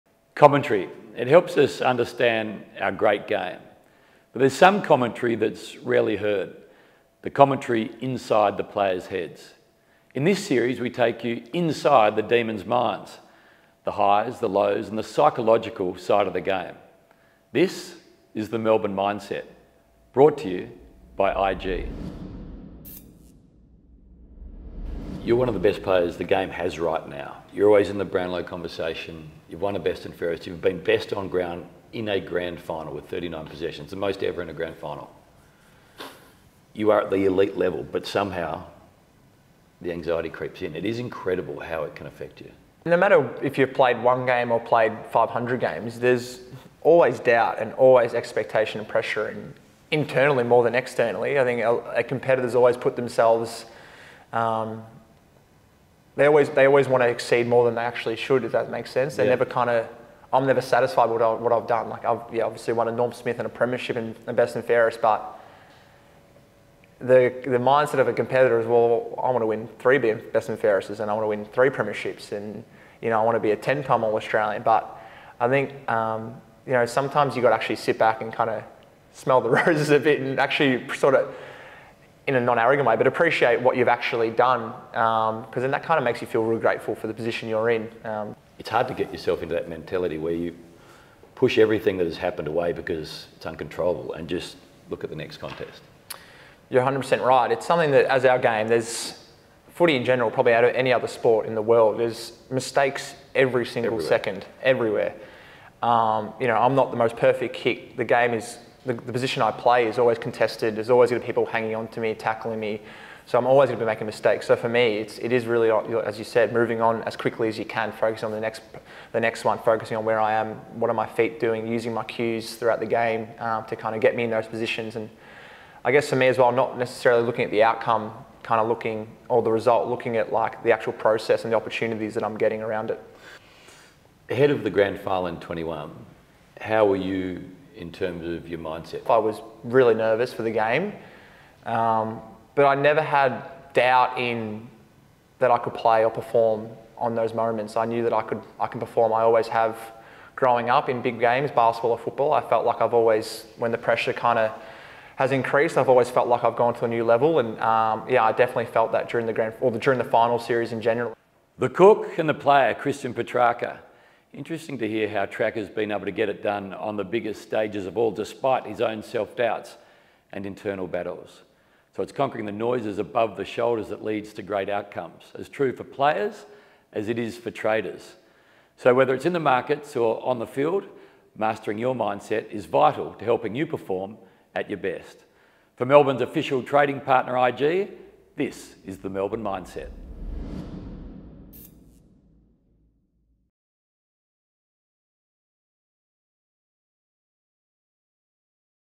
The Melbourne Mindset | Christian Petracca
In the third and final episode of our content series 'The Melbourne Mindset', Melbourne Football Club's Christian Petracca talks us through how he's learned to embrace self-doubt as a natural part of being a competitor.